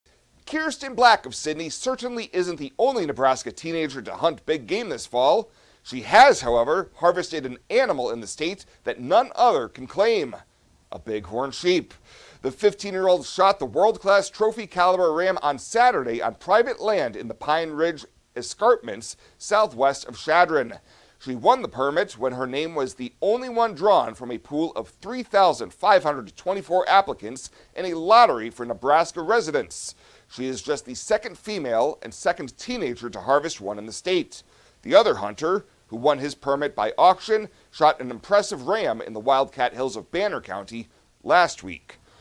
Nebraska Teen Bags Trophy Big Horn Sheep
Kiersten Black of Sidney certainly isn’t the only Nebraska teenager to hunt big game this fall. She has, however, harvested an animal in the state that none of the others can claim — a bighorn sheep.

Black, 15, shot the world-class trophy-caliber ram Saturday, Dec. 2, on private land in the Pine Ridge escarpments southwest of Chadron. She won the permit when her name was the only one drawn from a pool of 3,524 applicants in a lottery for Nebraska residents, which requires just a $29 application fee.

Black took to the field Saturday morning with father Trevor, younger brother Reed and family friend Tate Pieper.

They caught sight of the big ram with a group of ewes first thing that morning, but the sheep slipped into the trees and wasn’t spotted again until after about seven hours of searching the area and its steep canyons.

With her father’s coaching, Kiersten made a successful stalk and shot on the huge ram at sundown with just 25 minutes of legal shooting light to spare.

Todd Nordeen, the Nebraska Game and Parks Commission research and disease program manager who oversees the state’s bighorn sheep program, said the ram was 10½ years old — in the final stages of an average lifespan of 9-12 years.

Kiersten has developed an impressive hunting resume at a young age. The first of the five deer she has harvested was a whitetail buck with antlers of 150 inches – a size considered a once-in-a-lifetime achievement for most deer hunters. Earlier this year she shot a big mule deer in Colorado.

The teen said the bighorn sheep hunt was a thrilling experience, including the moment she learned her name was drawn.

“I was just so excited. I couldn’t believe it,” she said.

Trevor said he entered Kiersten’s name in the drawing, but made sure she knew of the commitment beforehand.

“You know what you’re signing up for if you draw? She said, ‘Oh, yeah, that sounds like fun.’ And here we are.

“You’ll be one of the only people eating bighorn sheep for Christmas dinner this year,” the father told his daughter as they sat near the ram.

Kiersten is a sophomore at Sidney High School. As a convenient coincidence, her Sidney Red Raider basketball team played at Chadron on the eve of the hunt. Had it been any other location, she surely would have sacrificed basketball for the once-in-a-lifetime hunt.

Kiersten’s harvest closes Nebraska’s 2023 season for bighorn sheep, in which just two permits were issued. The other hunter, who won his permit by auction, shot an impressive ram in the Wildcat Hills of Banner County earlier in the week.

The sheep mark the 33rd and 34th harvested in Nebraska since the Game and Parks’ hunting program began in 1998. Kiersten is just the second female and second teenager to harvest one in the state.

The number of Nebraska bighorn sheep permits available each year is based on the state’s population of the species, especially mature rams, as determined during monitoring by Game and Parks staff. To date, permits have been limited to one or two hunters in most years, with several years not permitting any harvests.

Nebraska has developed a reputation for producing trophy-caliber rams for those fortunate to win a permit. Not only do the hunts provide a rare experience and uncommon table fare, they have been vital to bighorn sheep conservation in Nebraska. About $2.5 million has been raised through the lottery applications and auctions to fund research and reintroduction efforts of the species.

Permit winners are assisted by Game and Parks staff and treated to meals and lodging at Fort Robinson State Park.

Nebraska’s reintroduction efforts for bighorn sheep began in the 1980s, an attempt to remedy the unregulated hunting, habitat loss and disease that led to their extirpation from the state in the late 1800s. Nebraska’s population of bighorns stands at approximately more than 250 rams, ewes and lambs in both the Wildcat Hills and Pine Ridge.